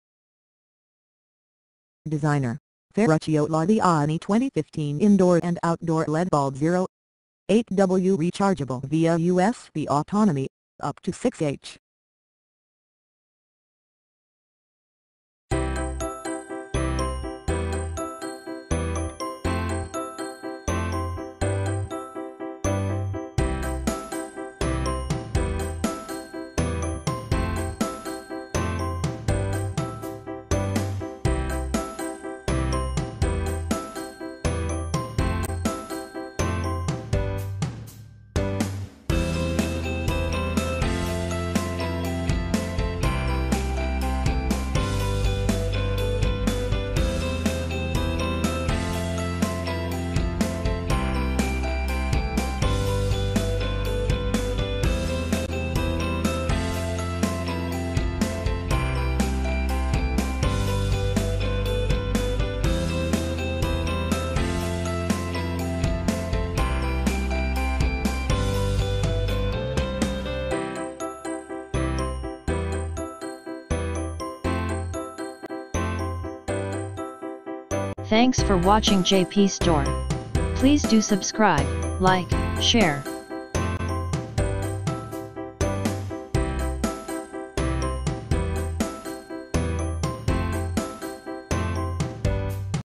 Kartell Battery Lampada da Tavolo, Cristallo [Classe di efficienza energetica A+]
Designer: Ferruccio Laviani 2015
    Indoor e Outdoor
    Lampadina LED 0.8 W
    Ricaricabile tramite USB
    Autonomia: fino a 6 h